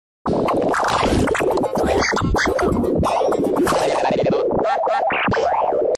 Klasky Csupo in Sad Waves (Earliest Version of VideoFactory/Vegas Movie Studio)
Created with Sonic Foundry VideoFactory 1.0 (Build 21) and Audacity 1.3.14 [using Windows ME on VMware Workstation 16.1.2 PRO].